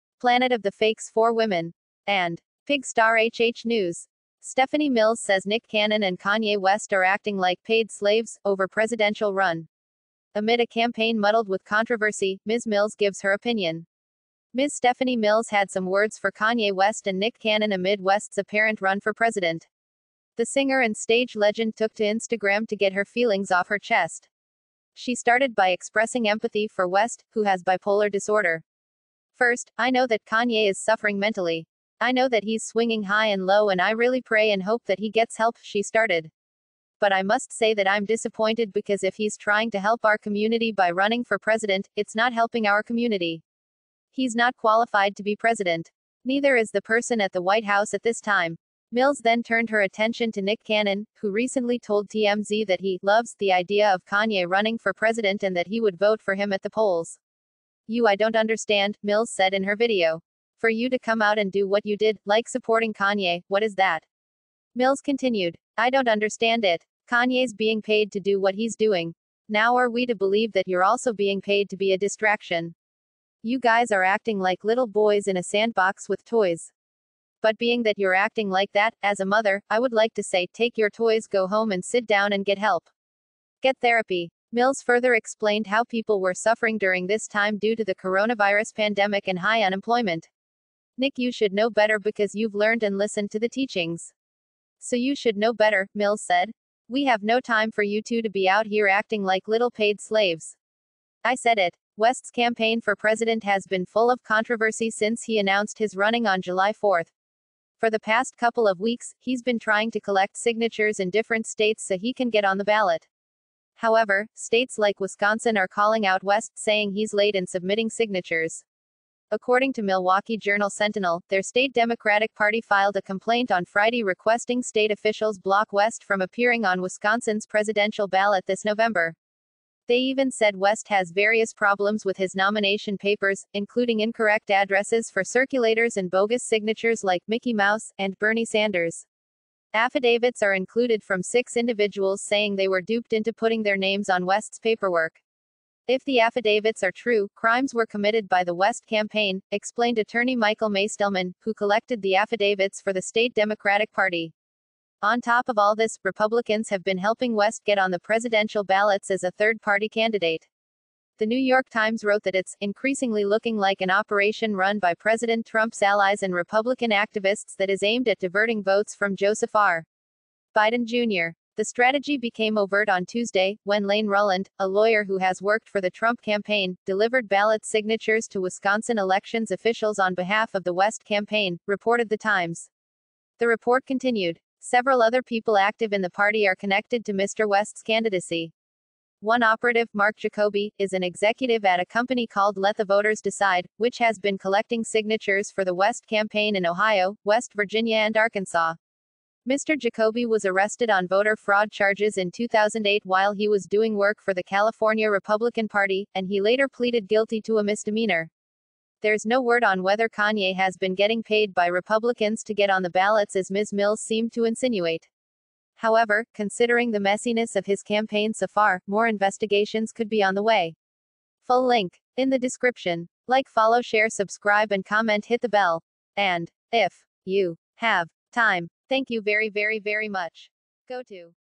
Stephanie Mills had some words for Kanye West and Nick Cannon
potfsHollywood:Ms. Stephanie Mills had some words for Kanye West and Nick Cannon "Acting like paid slaves"

Amid a campaign muddled with controversy, Ms. Mills gives her opinion.

The singer and stage legend took to Instagram to get her feelings off her chest. She started by expressing empathy for West, who has bipolar disorder.